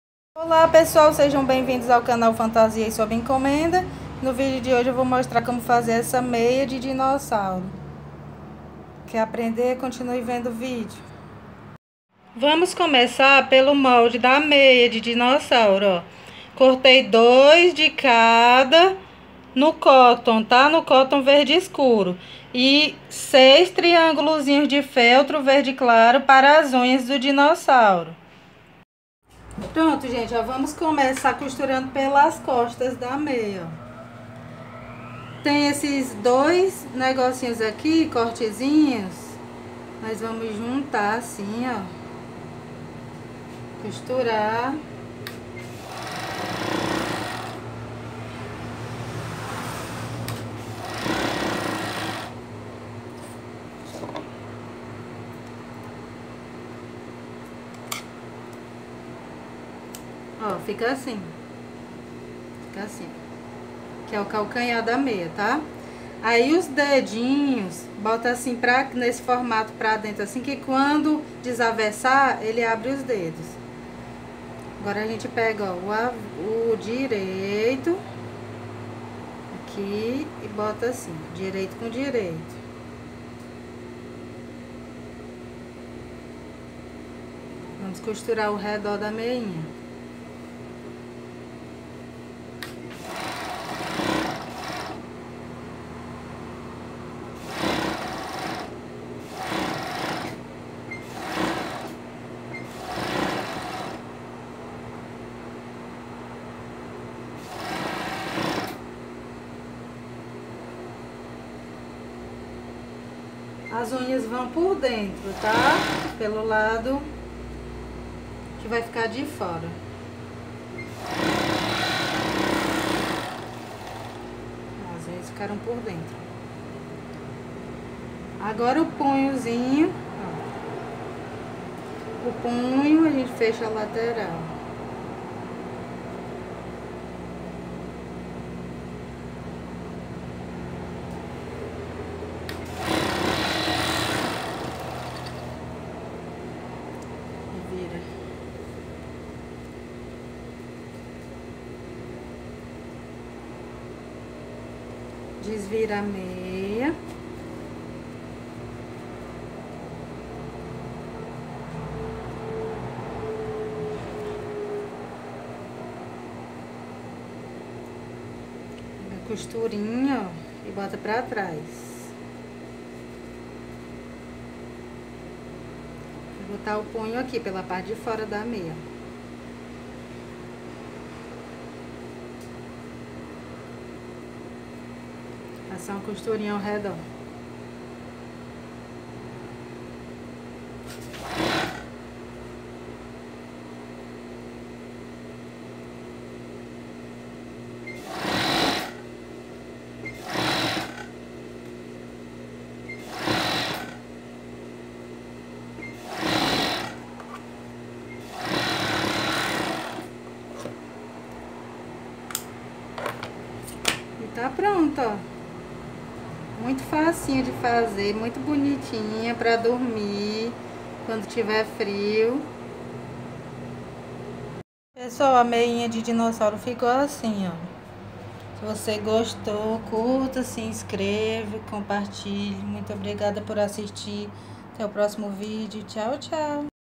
Como fazer meia de dinossauro 3 a 4 anos
Olá pessoal no vídeo de hoje iremos fazer uma meia de dinossauro para usar no frio para dormir!